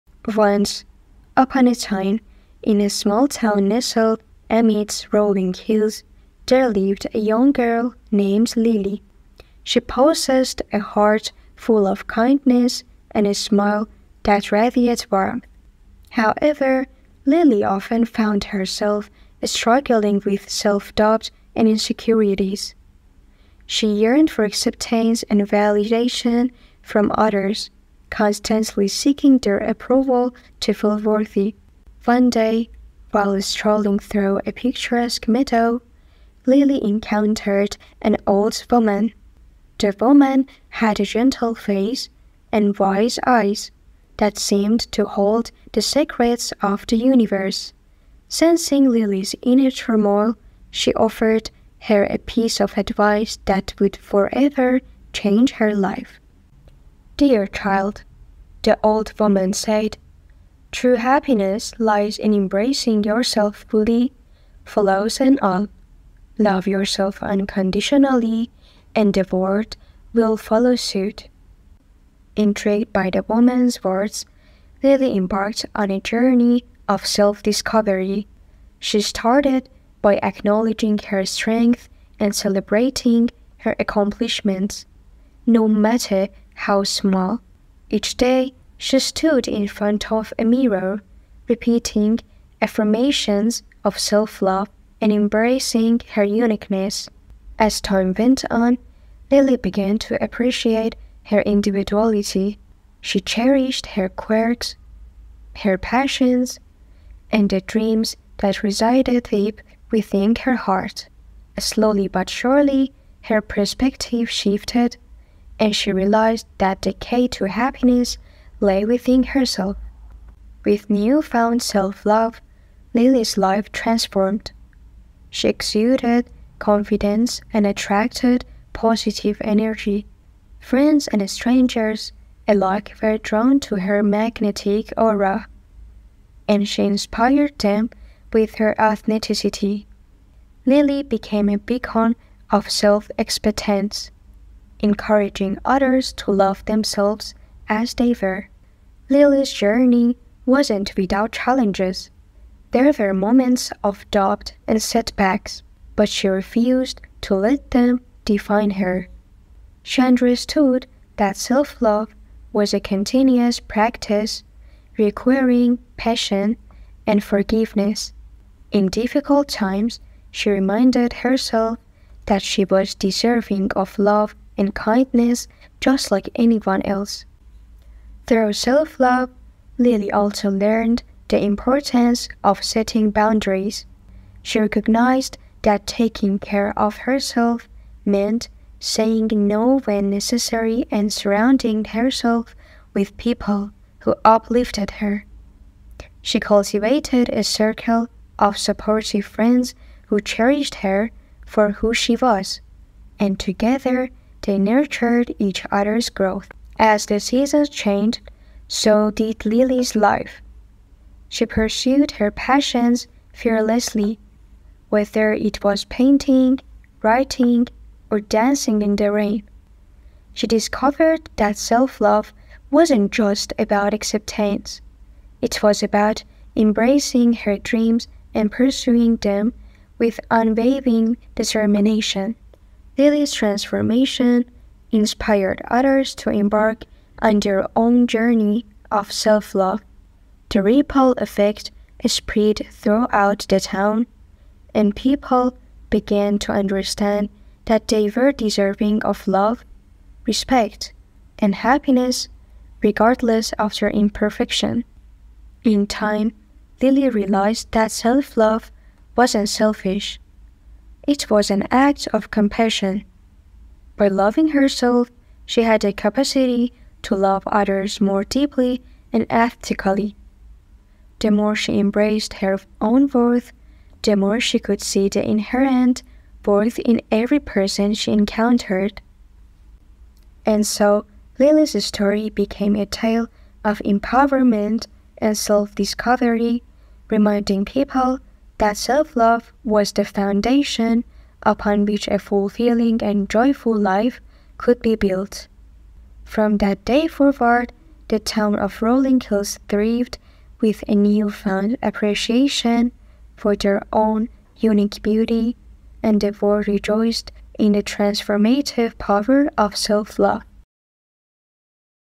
Improve Your English 👍 Self Love | Level 1 | English Story | #6
Embark on a captivating journey of self-discovery and language improvement with our unique video, "Journey to Self-Love." In this heartwarming tale, we intertwine the power of storytelling and the quest for English language enhancement.